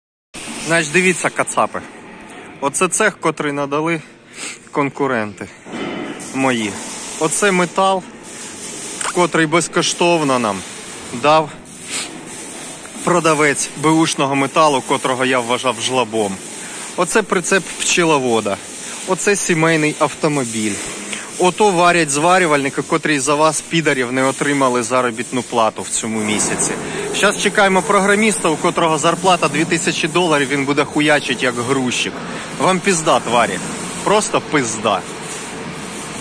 Вся суть УКРАЇНСЬКОЇ СИЛИ в одному відео
Починали самі й руками вантажили. На б/у металобазі зустріли людей з цехом й понеслося.

У воєнкоматі у командира очі на лоб полізли, коли просто зайшли якісь чуваки й запропонували їм закрити питання з протитанковими їжаками :)